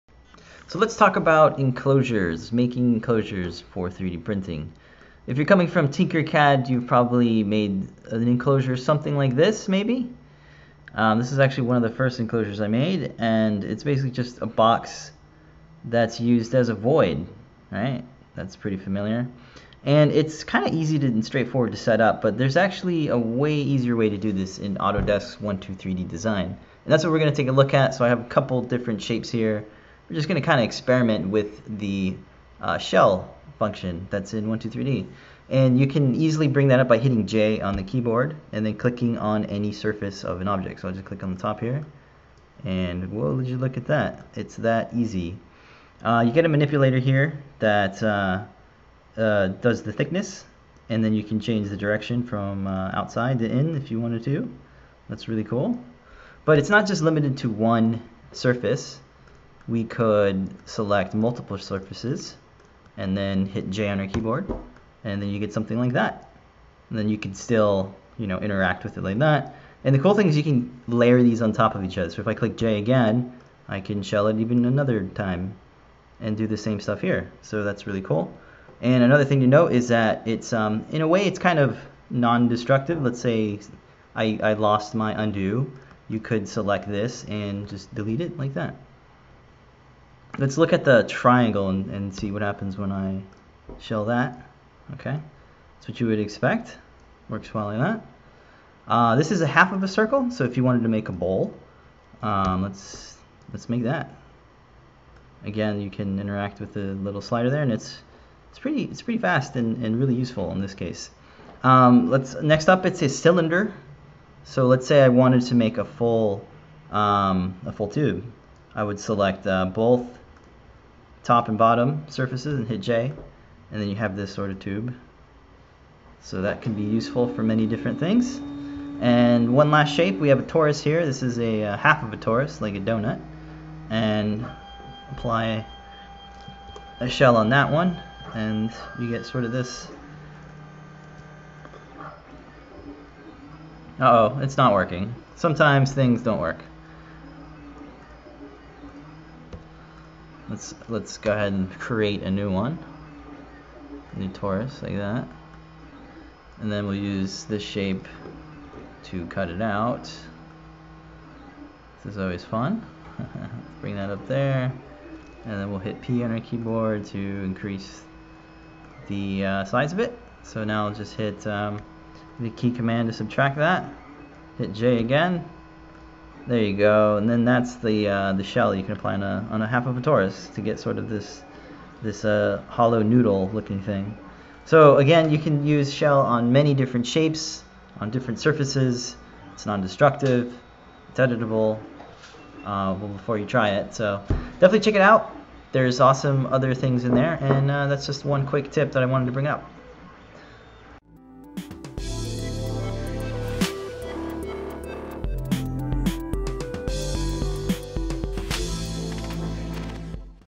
Make Hollow 3D Printed Models - CADurday
Using the shell feature in Autodesk 123D Design is a great way to hollow solid objects.

It's really easy to make enclosures by select a face on a cube and applying a shell. This works on all sorts of shapes. Spheres, maybe if you want to make a bowel. Cylinders, maybe you want to make a hollow tube.  The coolest thing you can do is select multiple surfaces to make a complex shell.

The shell feature isn't exactly parametric, nor is it completely destructive. Depending on how many features were applied to a given object, you can still remove the shell by selecting the surfaces and hitting the delete button on your keyboard. It's not editable once its been applied, but you can remove and reapply.